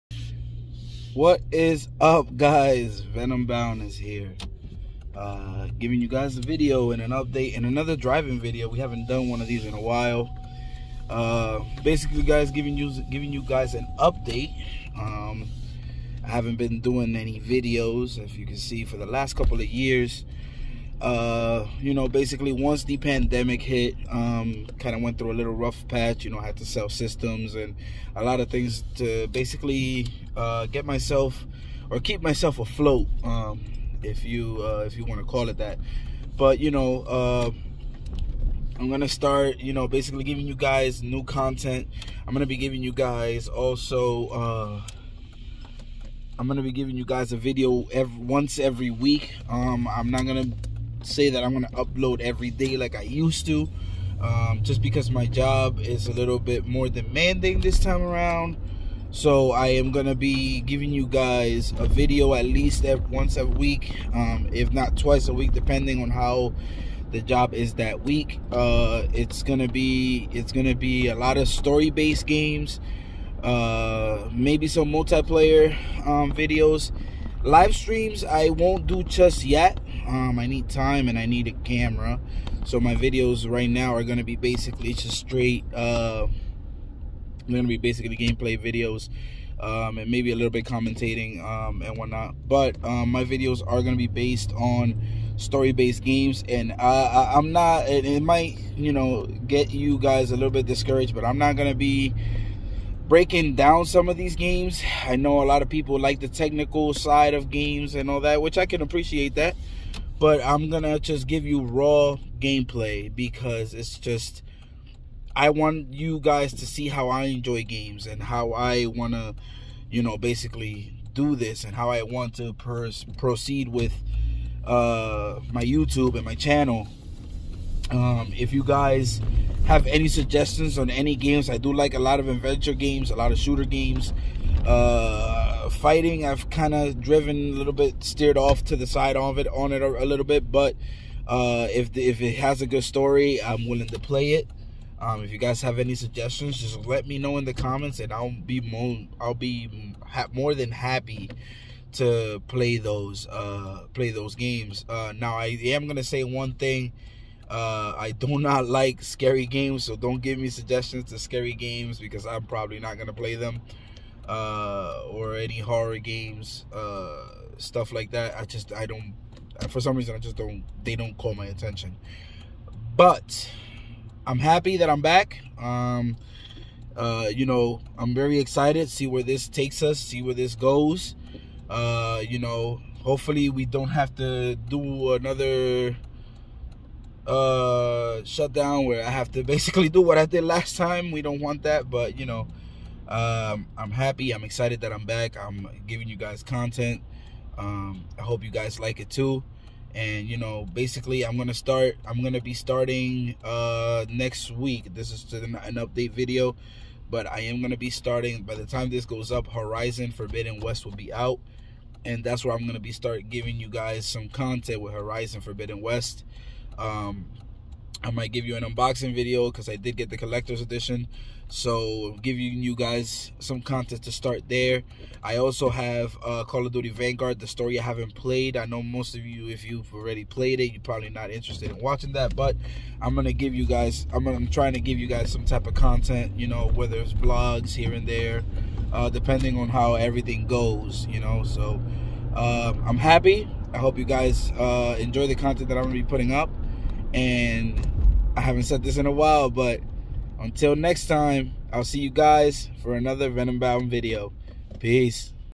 VENOMBOUND IS BACK!!!!!!
after two years I am finally in the right place and right state of mind to start putting content up again. thank you for you patience with me